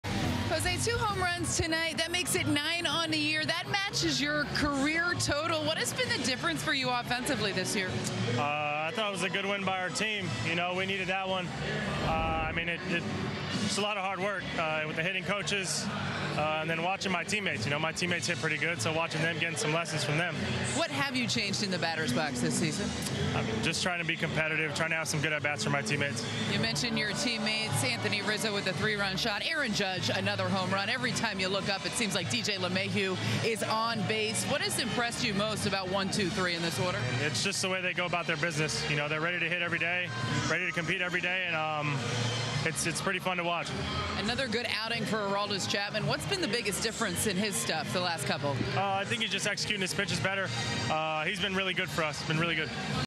Jose Trevino's first multi-HR night of career
Entering this season, Jose Trevino had 9 HR in his MLB career. His first multi-HR night gives the All-Star 9 this season with the Yankees!